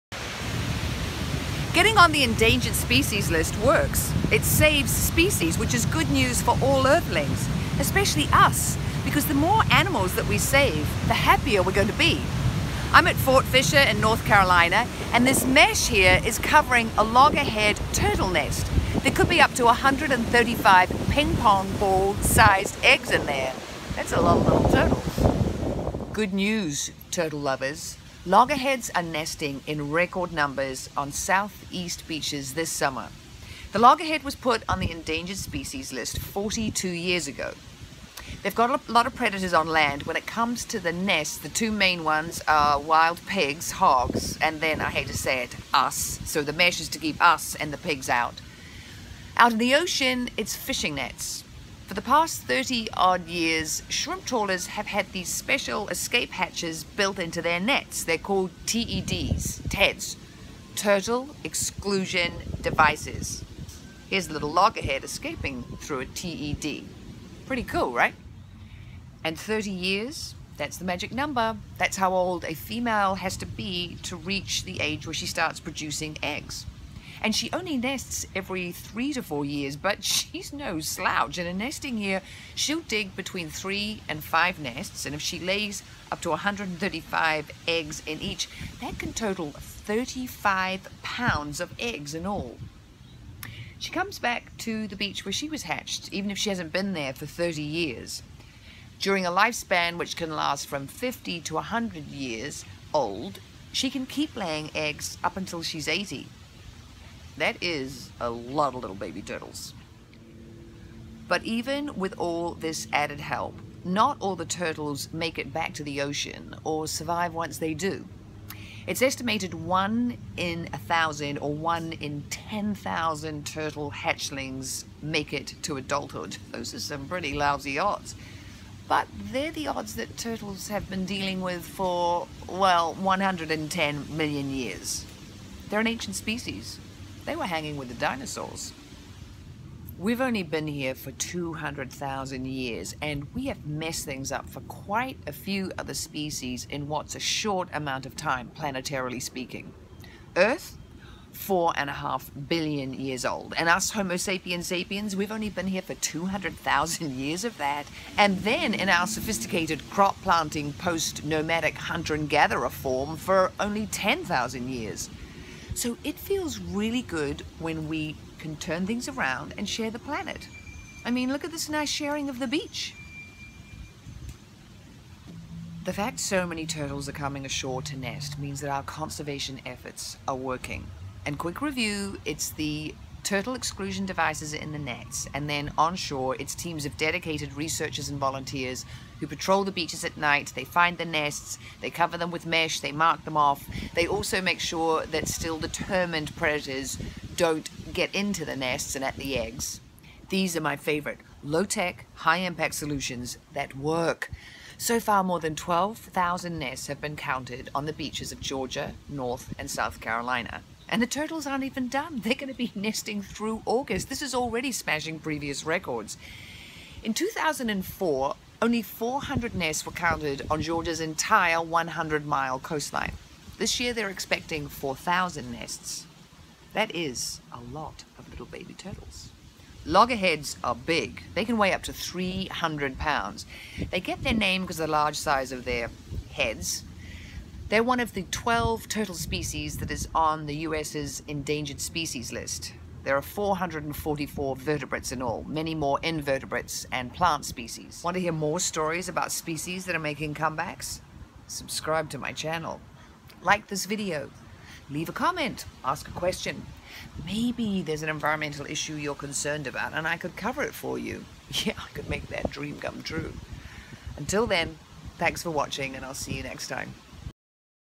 loggerhead turtles comeback
GOOD news for all earthlings: Loggerhead Turtles are making a comeback on southeast beaches! Protecting endangered species isn't just the right thing to do, maintaining biodiversity means a healthy functioning planet aka our life support system!

More reading:

U.S. Fish and Wildlife Services’ endangered reptiles list -